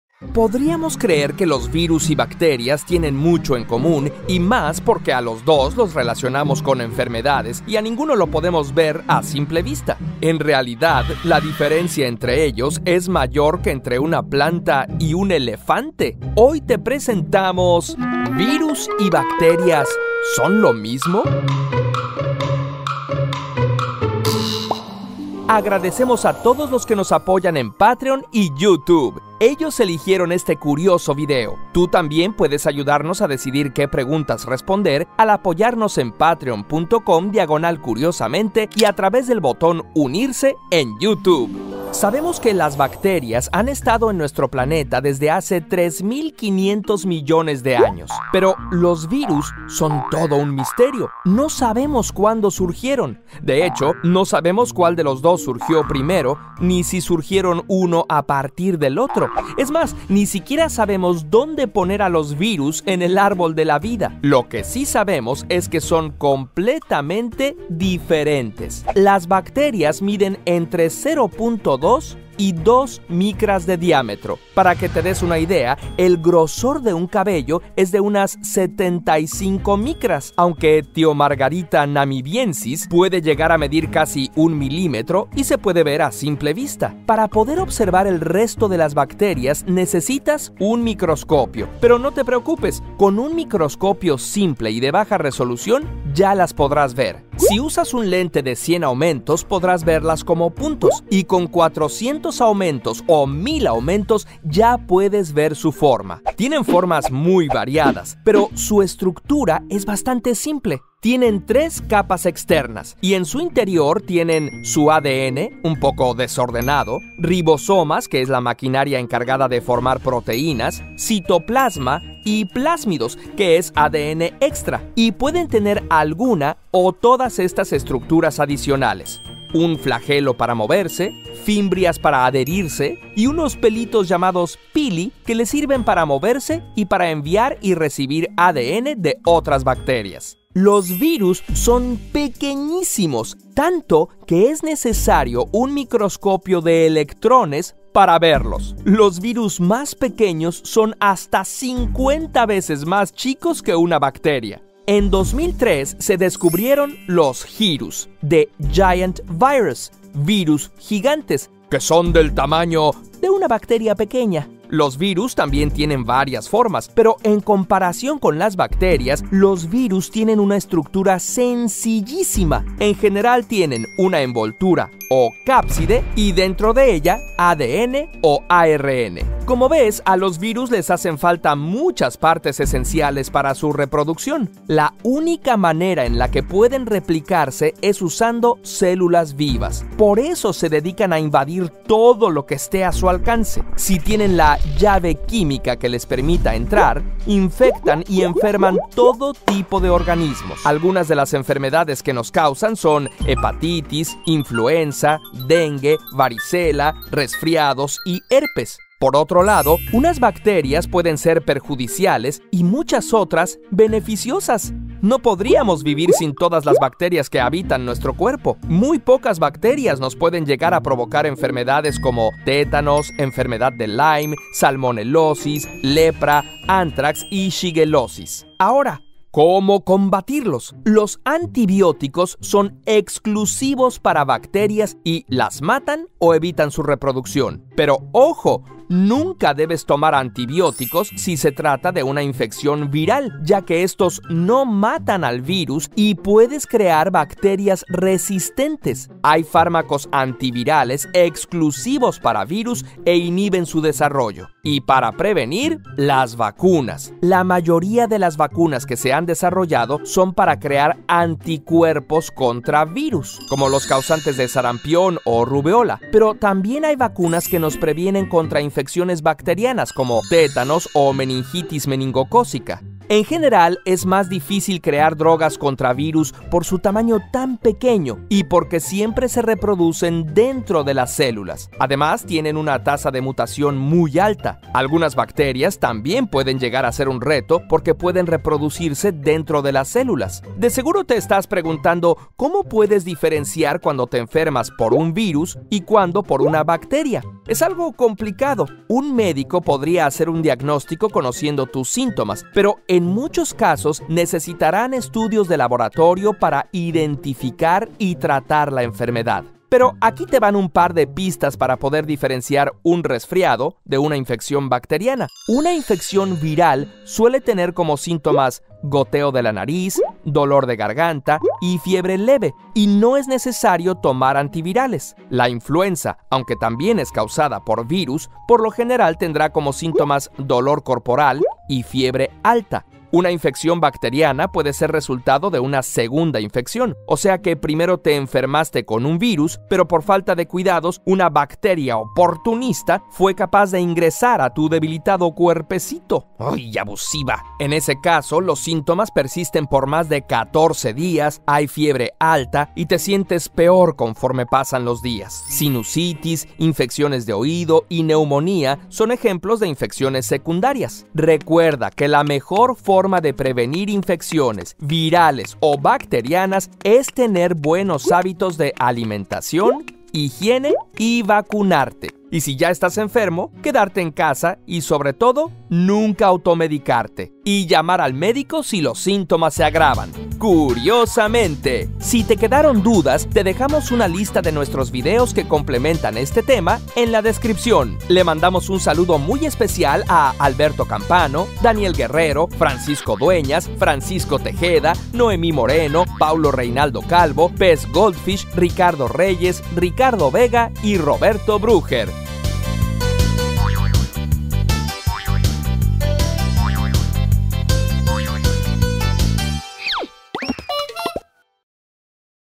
¿Cuál es la diferencia entre virus y bacterias?
Los dos son pequeñitos y siempre que pensamos en ellos pensamos en enfermedades, pero virus y bacterias son muy diferentes.

Guión: Mariana González
Ilustraciones: Érika Casab
Voz: Javier Lacroix